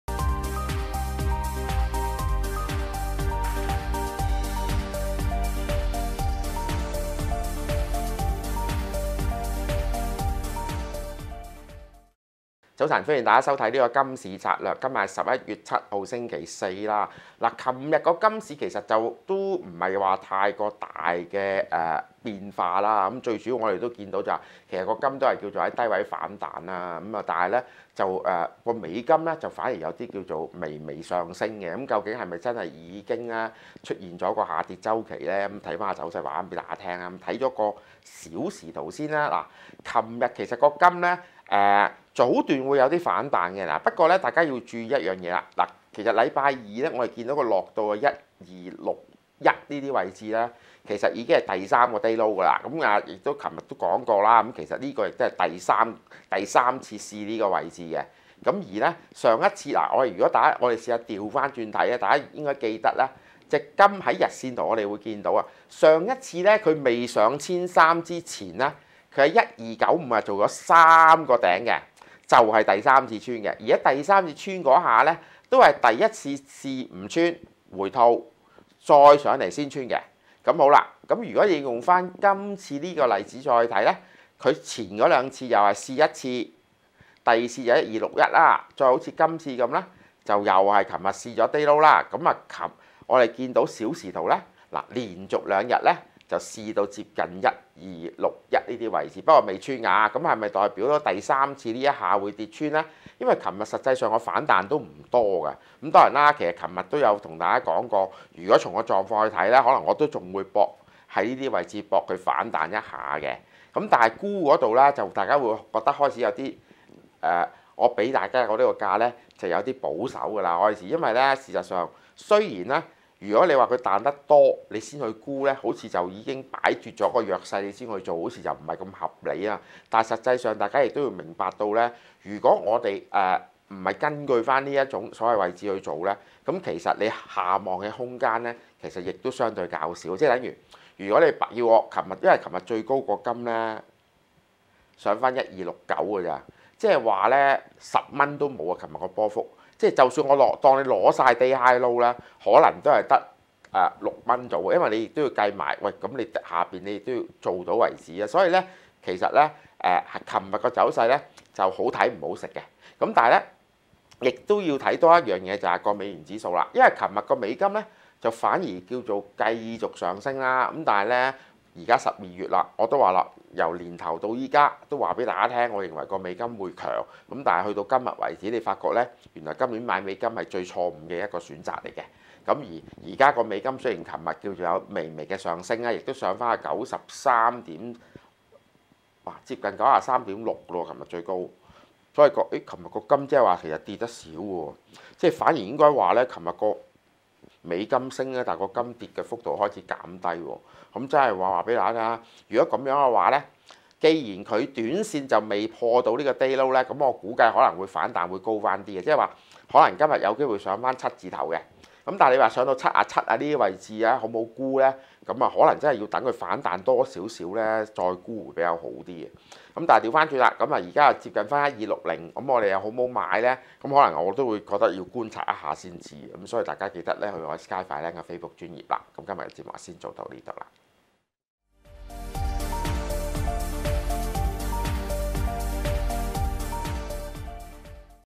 20171207 金市策略：大宗商品全面下跌（👇🏻留意文字簡介有即日限價盤策略）
即日策略：
入市價   買/賣   目標 1    目標 2    目標 3    止損
1261.00 買入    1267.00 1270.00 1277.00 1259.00
1253.00 買入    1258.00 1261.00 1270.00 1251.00
1271.00 賣出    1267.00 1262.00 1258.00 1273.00
1281.00 賣出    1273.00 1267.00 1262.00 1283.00